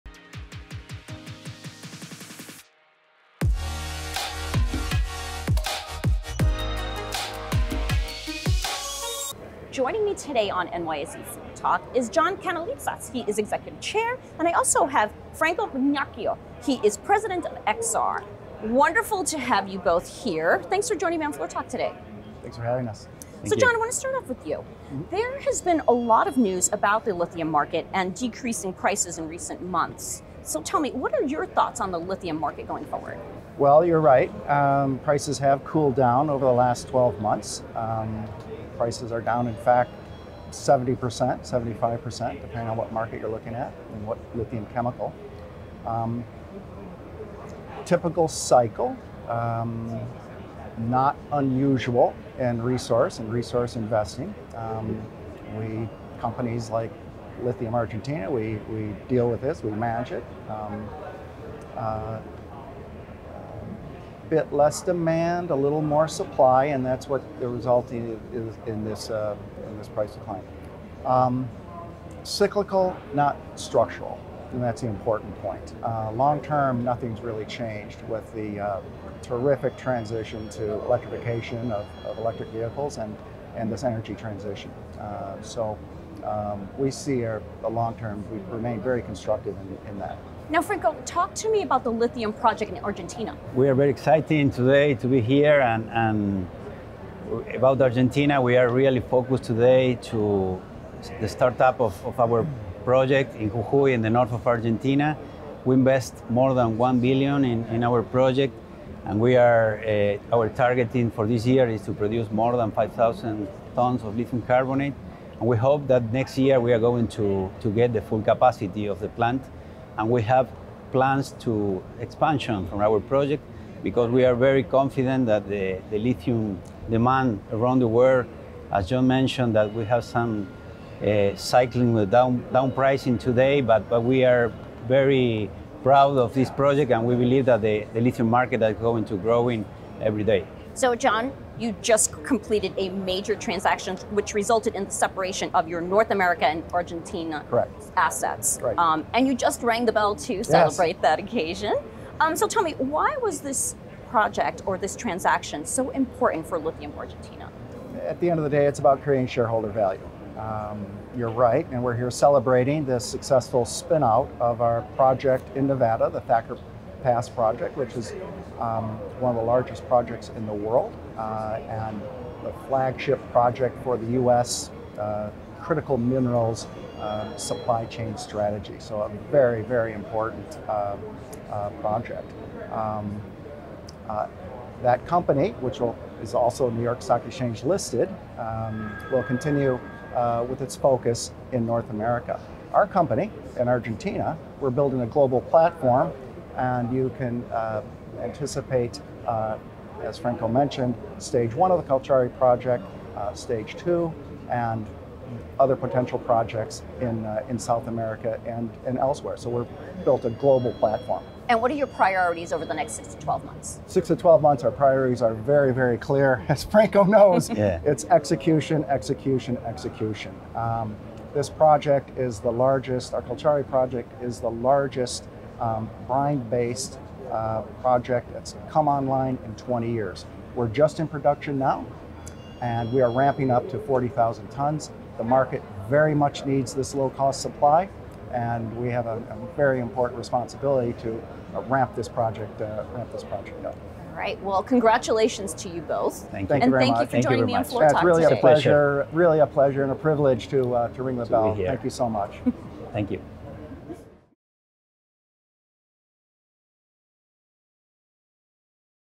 Lithium Argentina CEO discusses the current lithium market
John Kanellitsas, Executive Chair + Interim CEO at Lithium Argentina and Franco Mignacco, President of Exar Corporation, discuss the current lithium market and project in argentina on NYSEFloorTalk with Judy Khan Shaw.